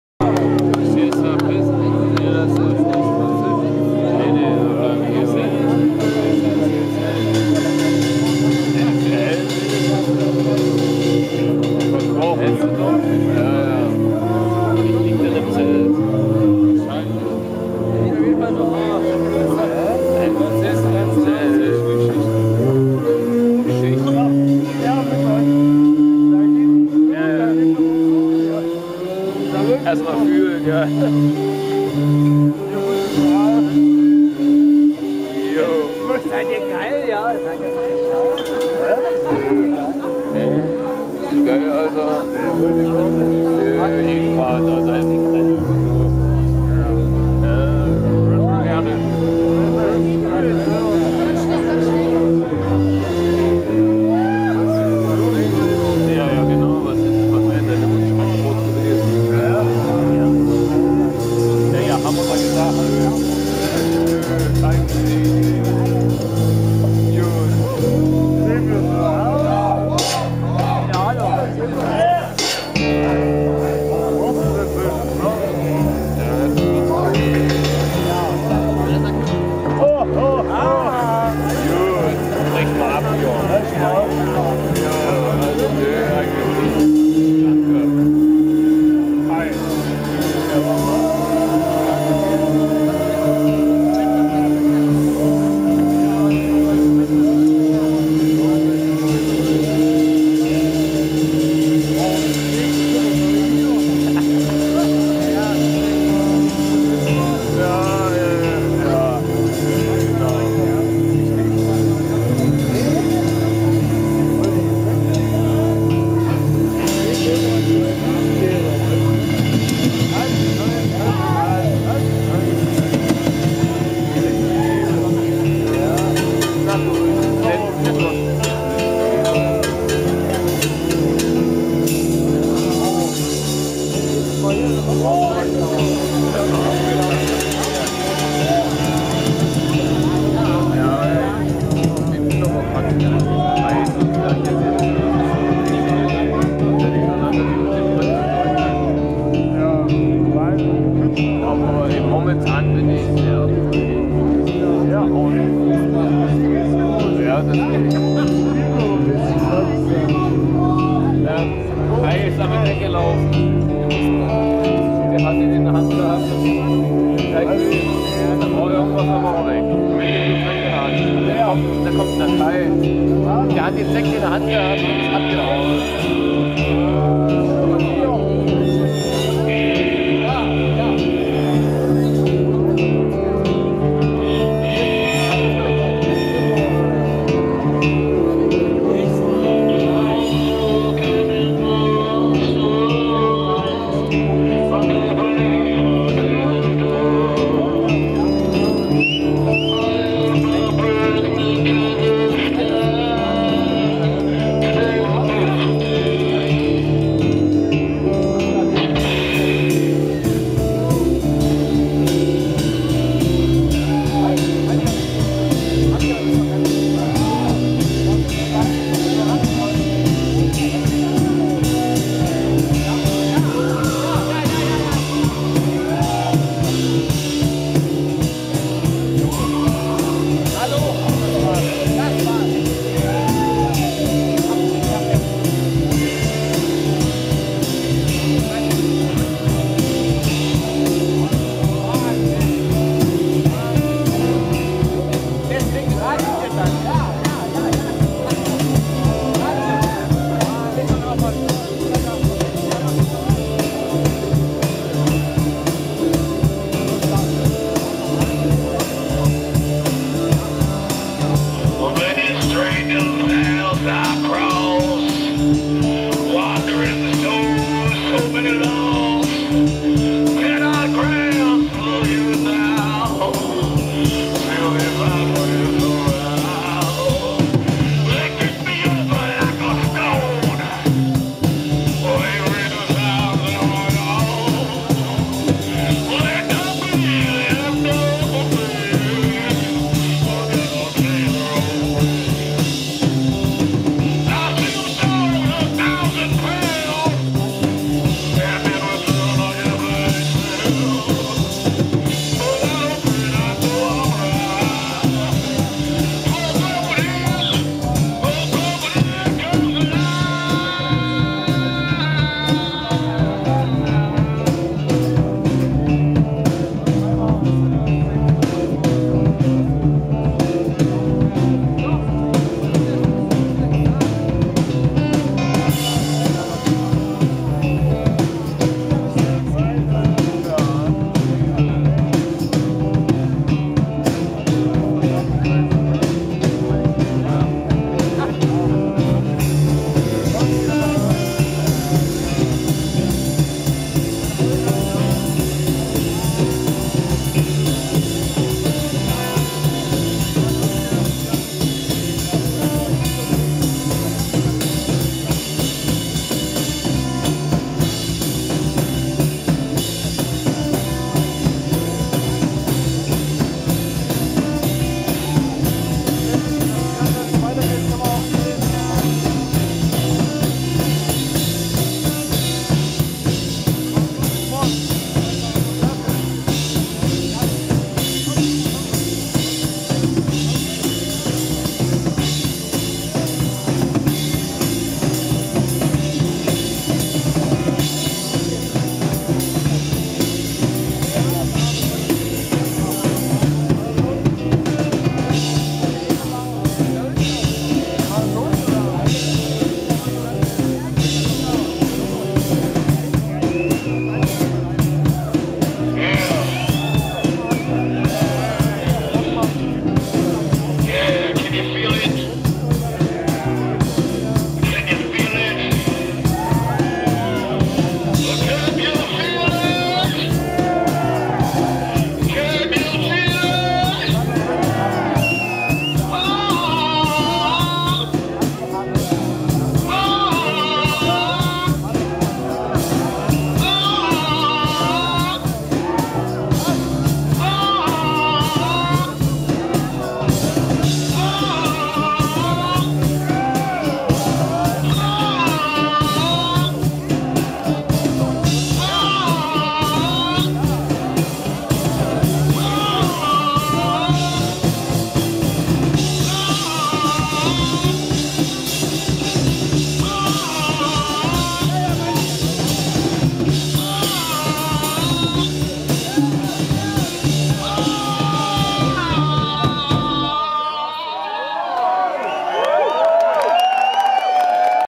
Reverend Shine Snake Oil Co @ Woodstock Forever, Waffenrod 16.8.2013
Das Woodstock Forever Festival in Waffenrod bietet für jeden Geschmack etwas an.  Reverend Shine Snake mit den Ursprüngen der amerikanischen Rock- und Popmusik: Blues, Jazz & Swing, Bluegrass & Folk, Gospel & Soul, Rocksteady & Afro.